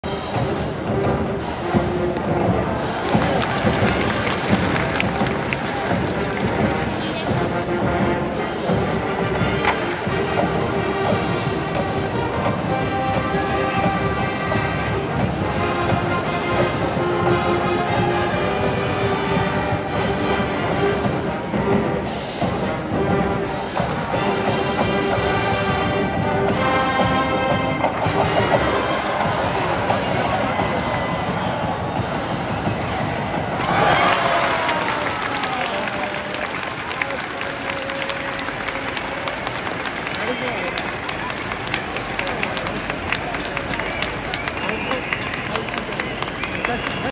沖縄尚学勝利の瞬間
第95回記念選抜高校野球大会
2023/03/25
沖縄尚学対クラーク記念国際
沖縄尚学、東恩納蒼、高橋歩希を
ファーストフライにうちとり、
3ー1で勝利。